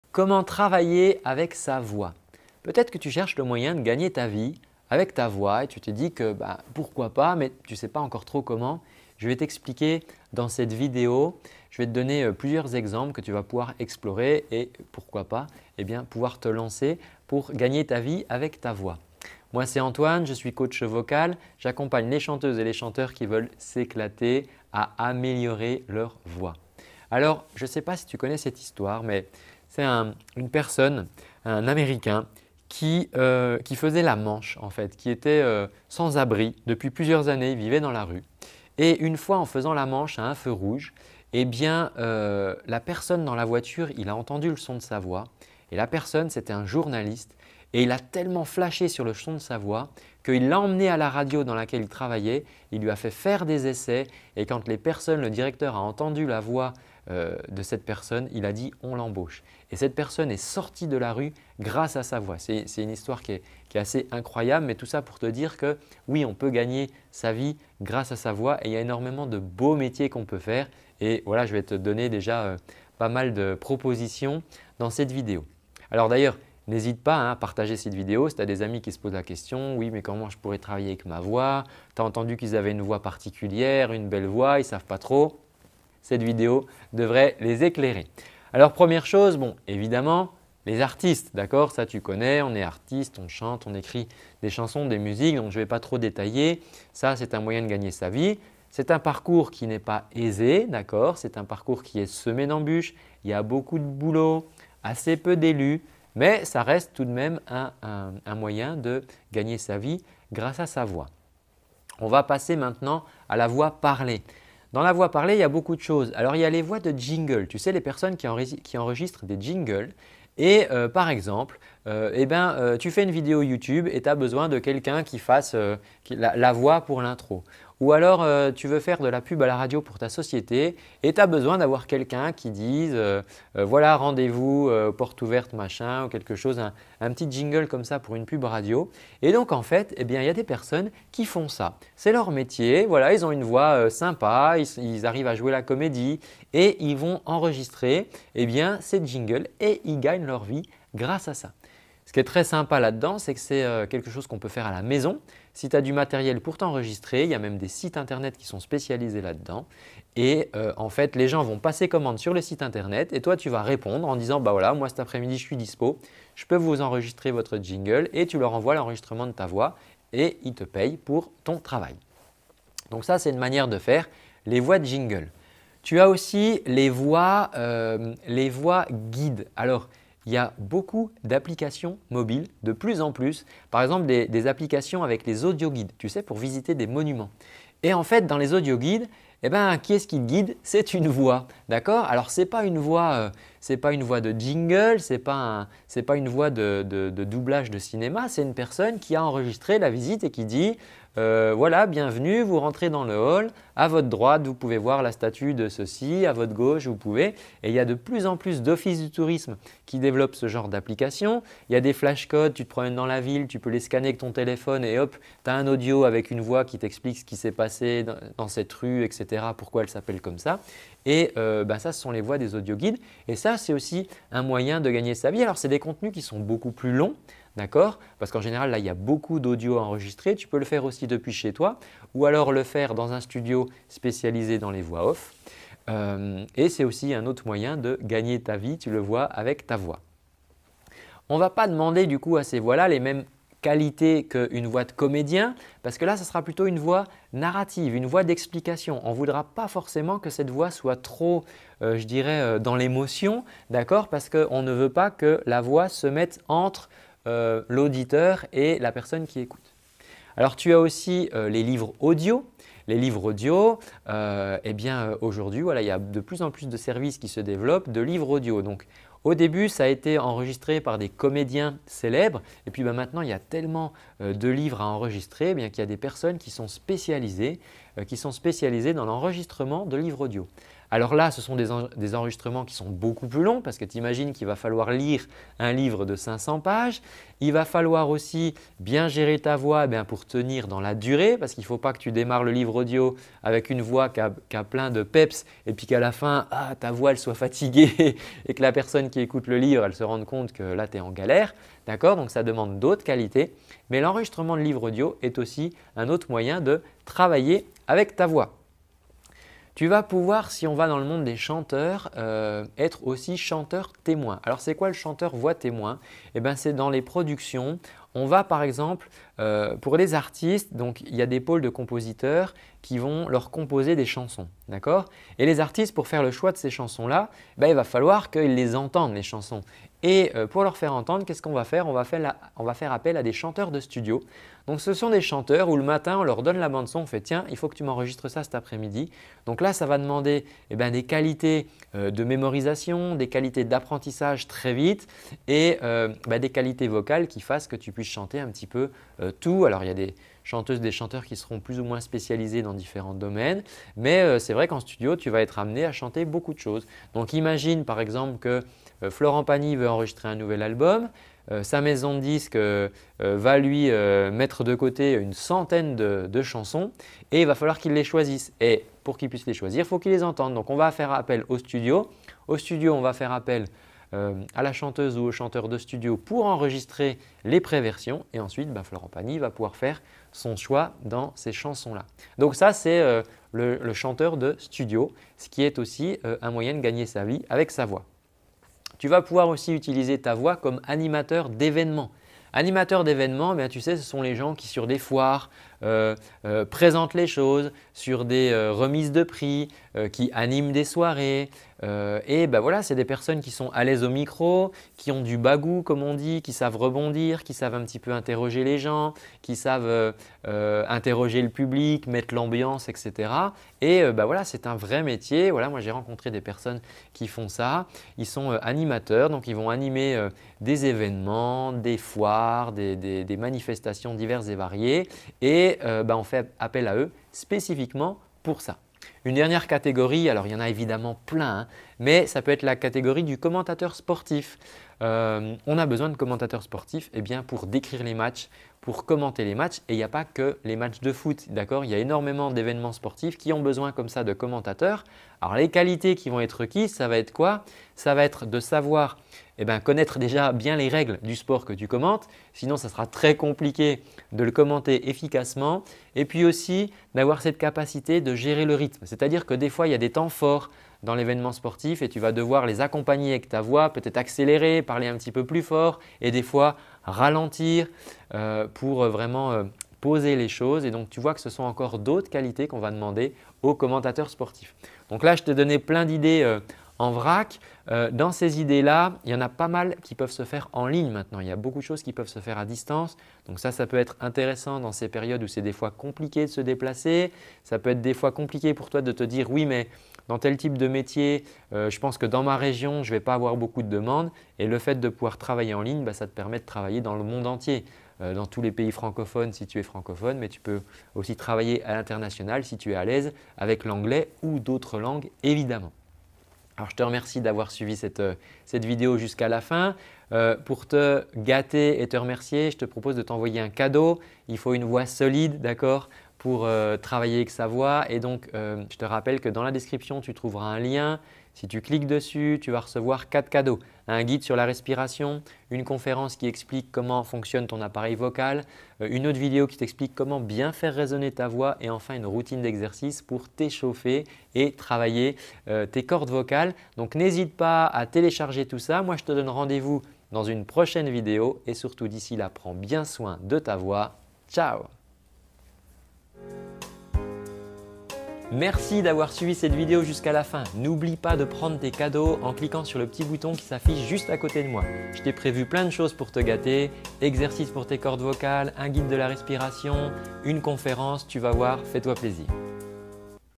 Comment travailler avec sa voix ?
Alors, premier métier, évidemment, c'est d'être artiste. Ça, tu connais. On est artiste, on chante, on écrit des chansons, des musiques. Je ne vais pas trop détailler. Ça, c’est un moyen de gagner sa vie. C’est un parcours qui n’est pas aisé. C’est un parcours qui est semé d’embûches. Il y a beaucoup de boulots, assez peu d’élus, mais ça reste tout de même un moyen de gagner sa vie grâce à sa voix.

On va passer maintenant à la voix parlée. Dans la voix parlée, il y a beaucoup de choses. Alors, il y a les voix de jingles. Ce qui est très sympa là-dedans, c’est que c’est quelque chose qu’on peut faire à la maison si tu as du matériel pour t’enregistrer. Il y a même des sites Internet qui sont spécialisés là-dedans. En fait, les gens vont passer commande sur le site internet.

Tu as aussi les voix guides. Alors, il y a beaucoup d’applications mobiles de plus en plus, par exemple des applications avec les audioguides pour visiter des monuments.

Alors, tu as aussi les livres audio. Les livres audio, aujourd’hui, il y a de plus en plus de services qui se développent, de livres audio. Au début, ça a été enregistré par des comédiens célèbres. Maintenant, il y a tellement de livres à enregistrer qu’il y a des personnes qui sont spécialisées dans l’enregistrement de livres audio.

Tu vas pouvoir, si on va dans le monde des chanteurs, être aussi chanteur témoin. C’est quoi le chanteur voix témoin ? Dans les productions, on va par exemple pour les artistes, il y a des pôles de compositeurs qui vont leur composer des chansons. Les artistes pour faire le choix de ces chansons-là, il va falloir qu’ils les entendent les chansons. Pour leur faire entendre, qu’est-ce qu’on va faire ? On va faire appel à des chanteurs de studio. Ce sont des chanteurs où le matin, on leur donne la bande-son. On fait, il faut que tu m’enregistres ça cet après-midi. Donc là, ça va demander des qualités de mémorisation, des qualités d’apprentissage très vite et des qualités vocales qui fassent que tu puisses chanter un petit peu tout. Il y a des chanteuses et des chanteurs qui seront plus ou moins spécialisés dans différents domaines. Mais c’est vrai qu’en studio, tu vas être amené à chanter beaucoup de choses.

Tu vas pouvoir aussi utiliser ta voix comme animateur d’événements. Animateur d’événements, tu sais, ce sont les gens qui, sur des foires, présentent les choses, sur des remises de prix, qui animent des soirées.

Une dernière catégorie. Alors, il y en a évidemment plein, mais ça peut être la catégorie du commentateur sportif. On a besoin de commentateur sportif pour décrire les matchs, pour commenter les matchs et il n’y a pas que les matchs de foot. Il y a énormément d’événements sportifs qui ont besoin de commentateur.

Alors les qualités qui vont être requises, ça va être quoi ? Ça va être de savoir connaître déjà bien les règles du sport que tu commentes, sinon ce sera très compliqué de le commenter efficacement. Puis aussi d’avoir cette capacité de gérer le rythme.

Donc là, je t’ai donné plein d’idées en vrac. Dans ces idées-là, il y en a pas mal qui peuvent se faire en ligne maintenant. Il y a beaucoup de choses qui peuvent se faire à distance. Donc ça, ça peut être intéressant dans ces périodes où c’est des fois compliqué de se déplacer.

Le résultat ?